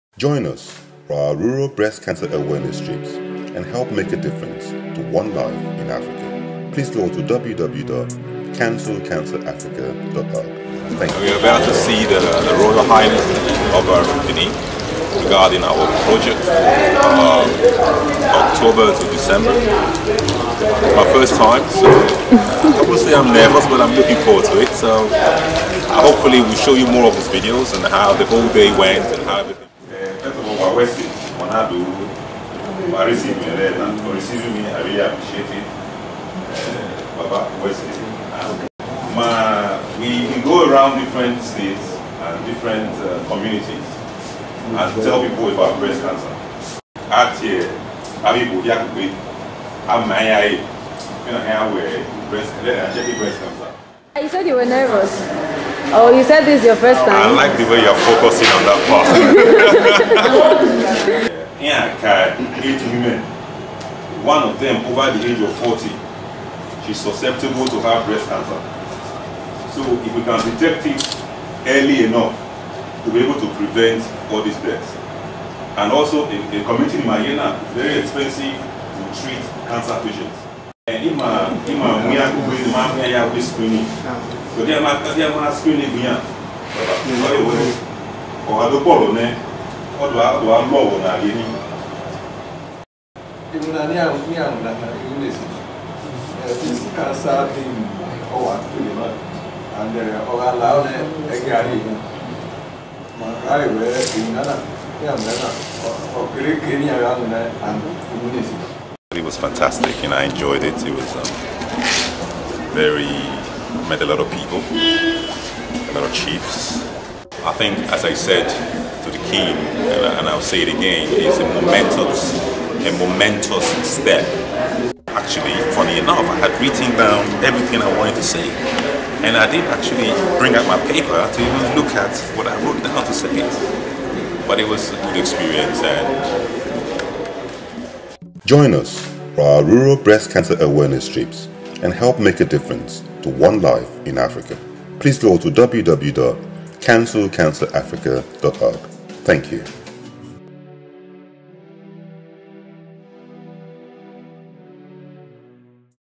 Ep 16 - Our visit to the Edo King.
The Day we visited the KIng of Benin Kingdom.

Before we started our Breast Cancer awareness program in Edo State, we visited the King to get his blessings.

It was a successful visit and a day we all will always remember. I wish to thank him for his support and prayers.

The Breast Cancer Awareness program was also a success and we look forward to our yearly visit to the community.

Please continue to support our efforts. To give to us online please go